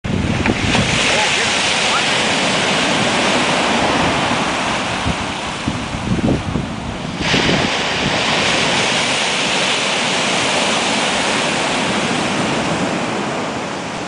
Icy Ocean
Icy, icy ocean at Rehoboth Beach Taken on February 28, 2015